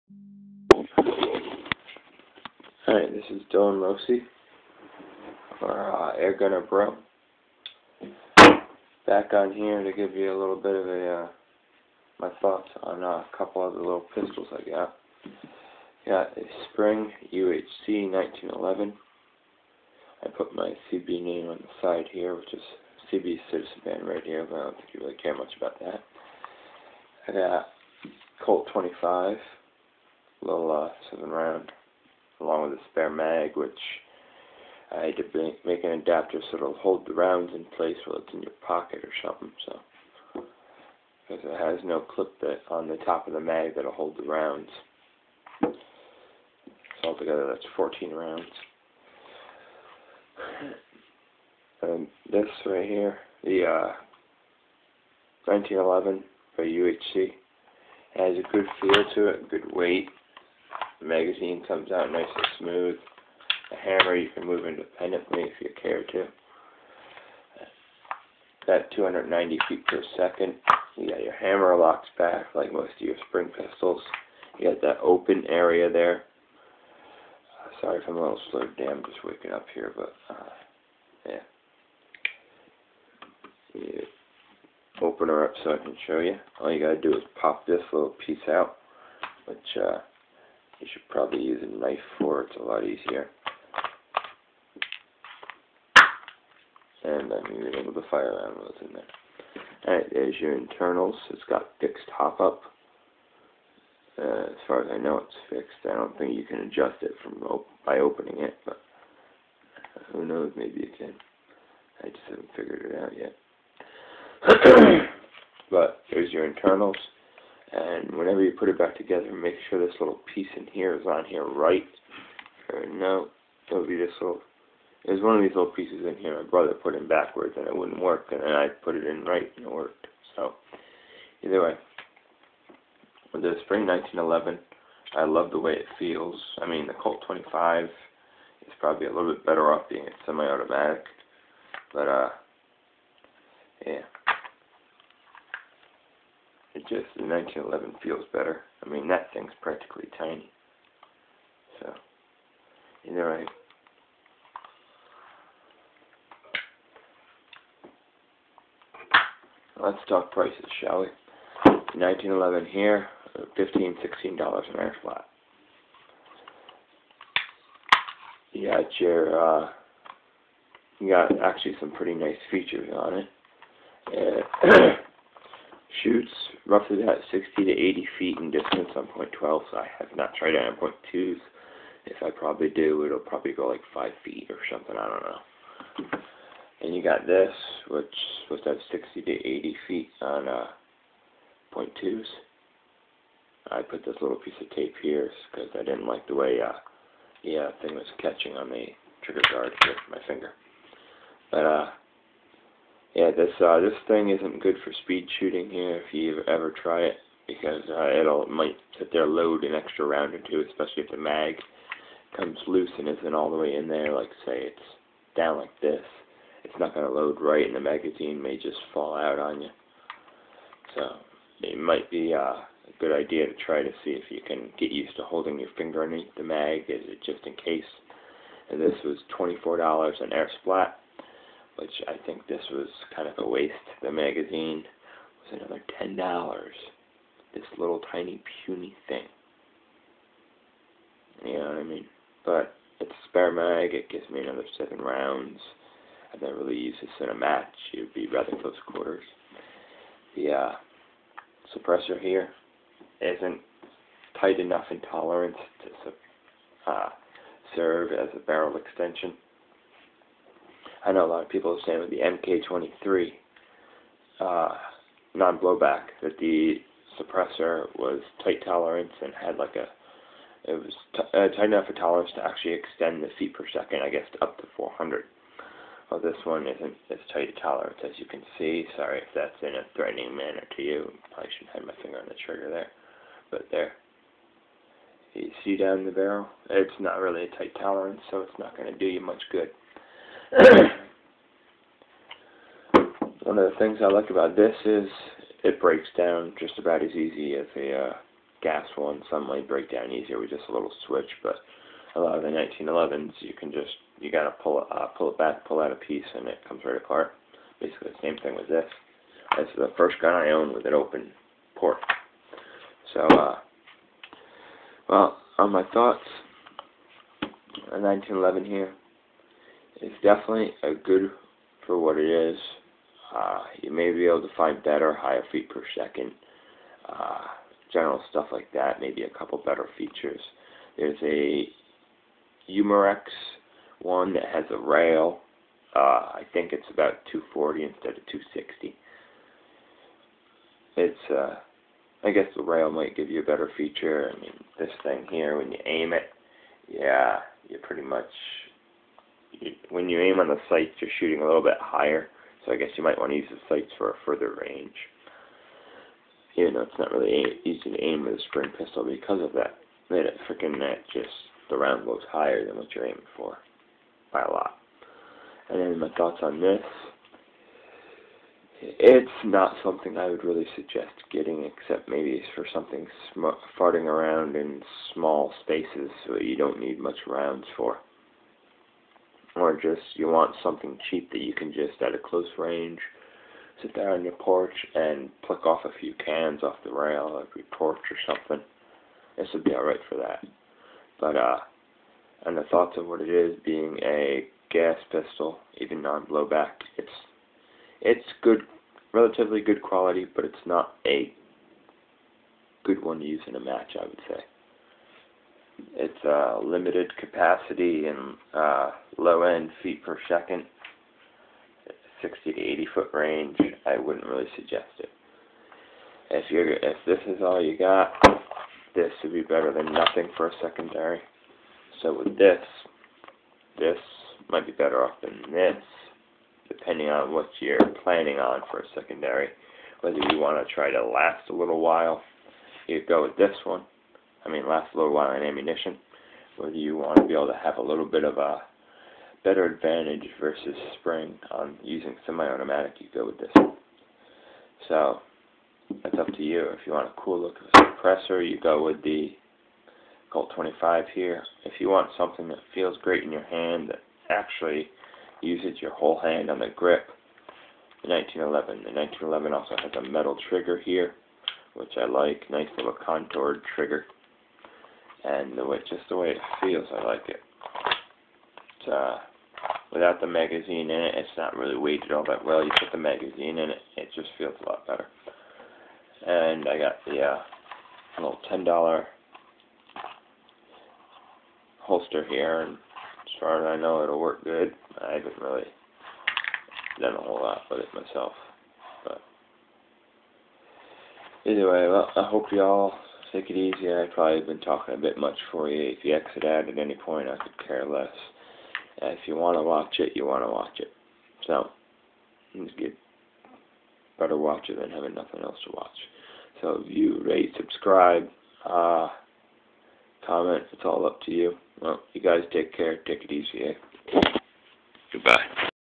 My thaughts on the UHC 1911 and the colt 25
I also disasembled my spring pistol to give you an inside look ps bolth pistols have fixed hop up